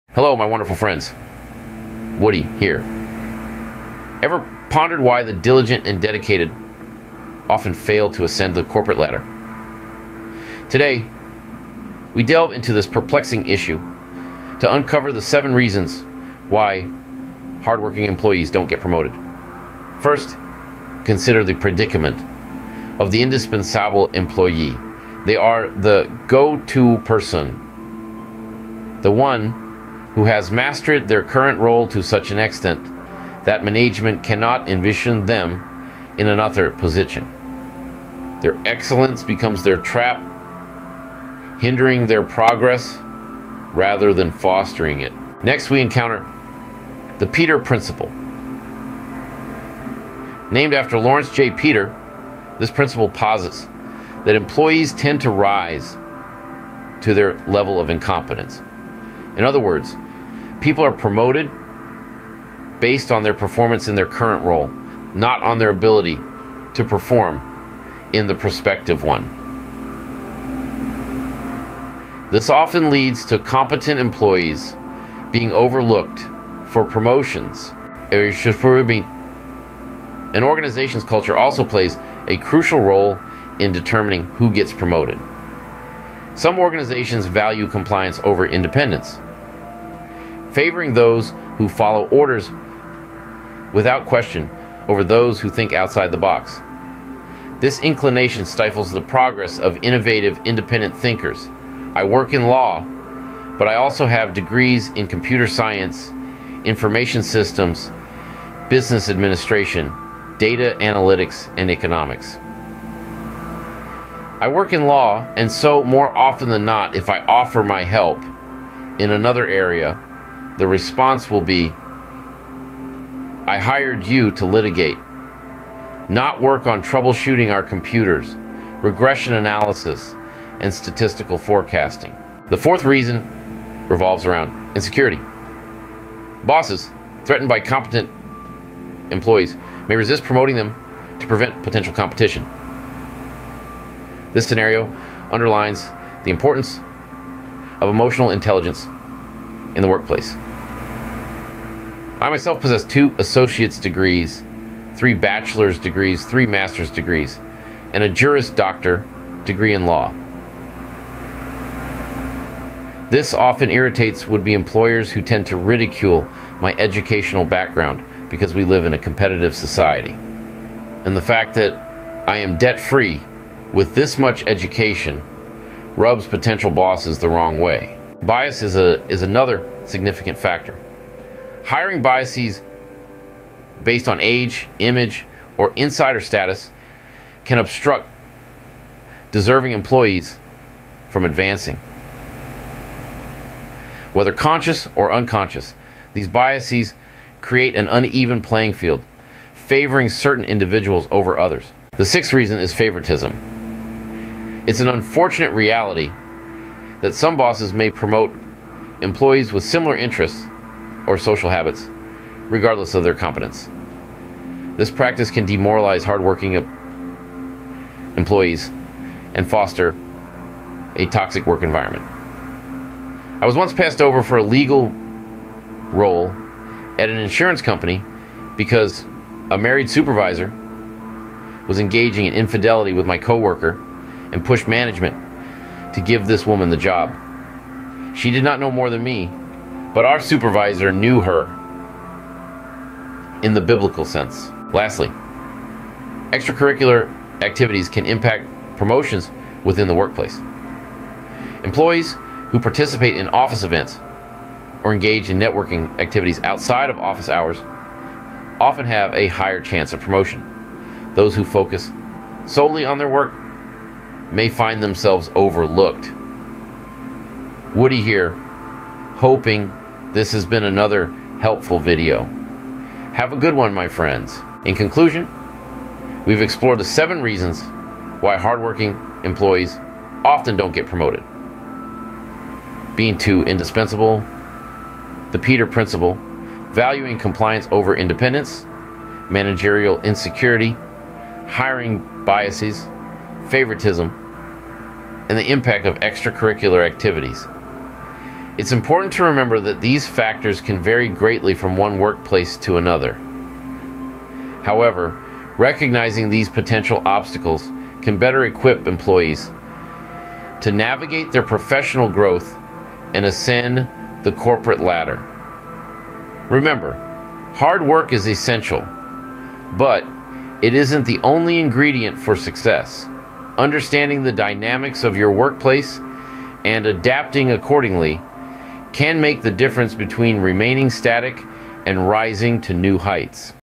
Can't seem to get promoted at work?
The Hidden Traps of Corporate Ladders and the Meritocracy Myth

OUTLINE:
00:00:00
The Trap of Indispensability

00:00:45
The Peter Principle

00:01:17
Cultural Influence

00:02:12
Managerial Insecurity

00:02:58
Bias in Promotion

00:03:21
The Dark Side of Favoritism

00:04:04
Impact of Extracurricular Activities

00:04:34
Overcoming Obstacles